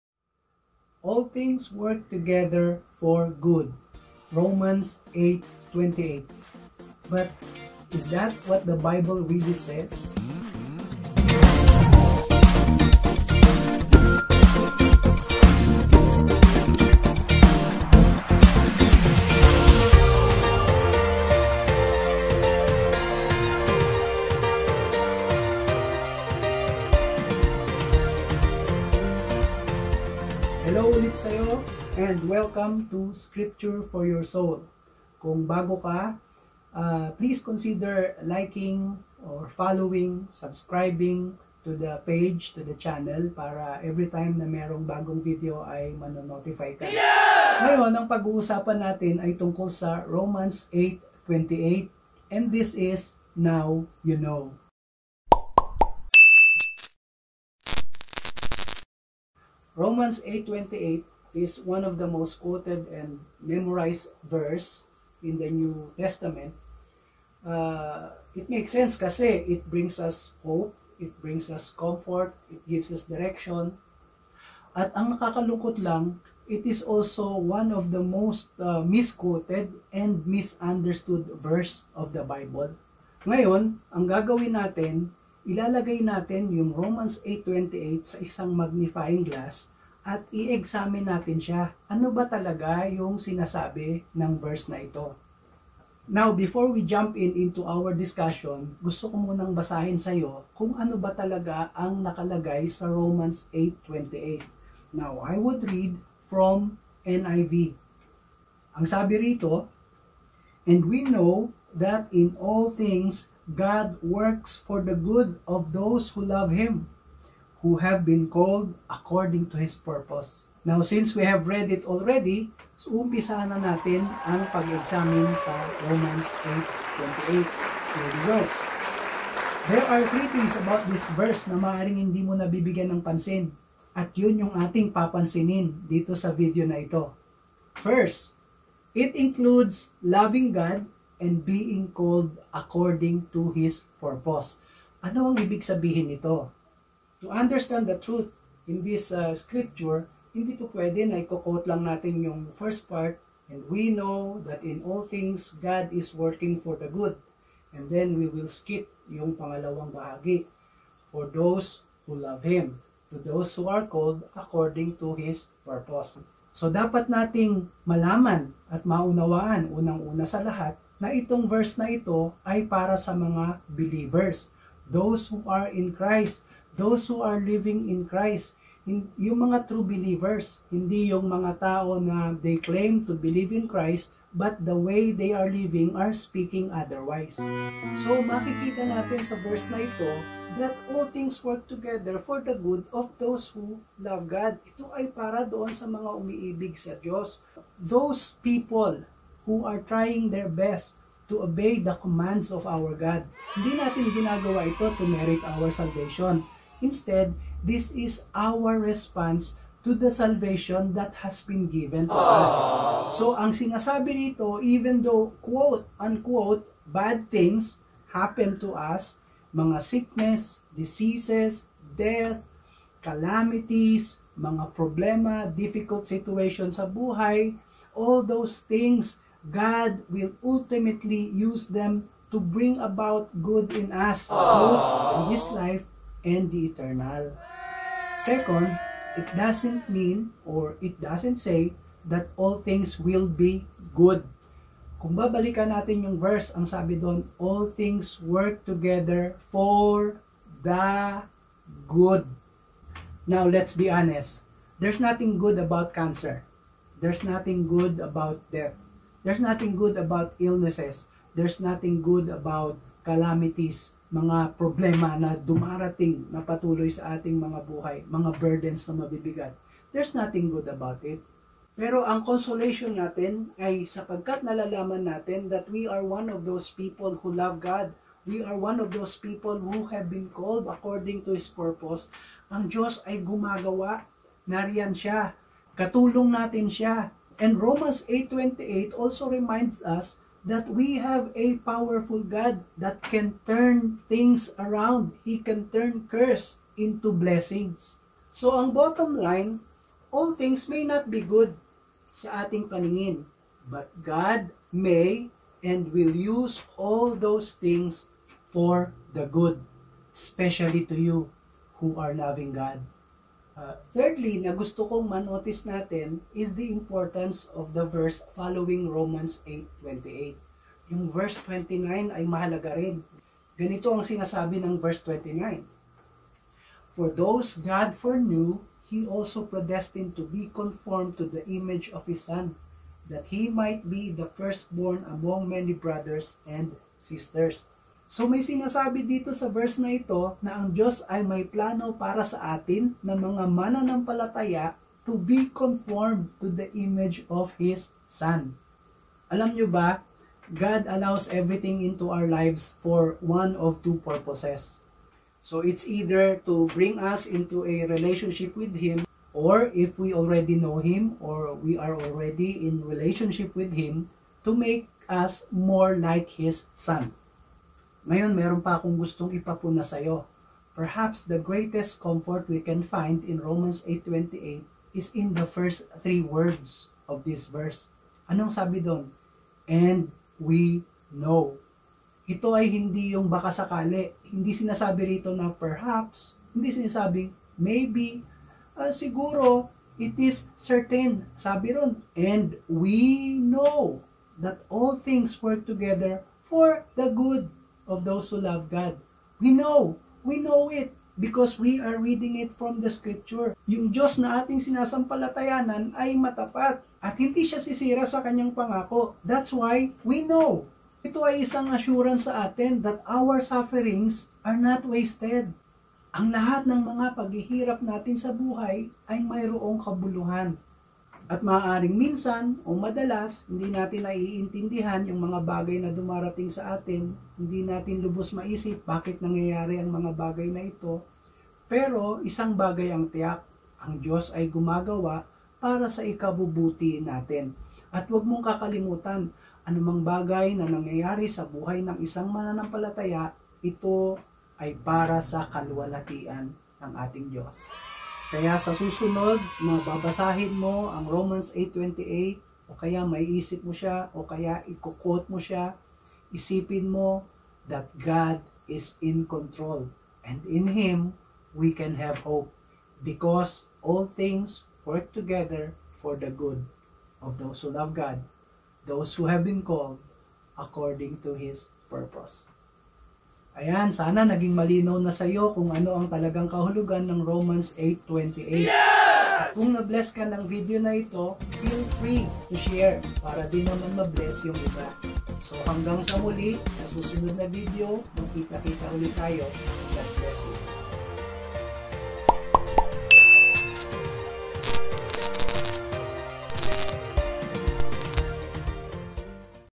Now You Know Vid-2 | 𝐀𝐋𝐋 𝐓𝐇𝐈𝐍𝐆𝐒 𝐖𝐎𝐑𝐊 𝐓𝐎𝐆𝐄𝐓𝐇𝐄𝐑 𝐅𝐎𝐑 𝐆𝐎𝐎𝐃... 𝐑𝐄𝐀𝐋𝐋𝐘?
Examining Romans 8:28
In reading and studying the Bible, context plays an important role. Understanding passages of Scripture in it's right context can avoid confusion, deception and wrong application.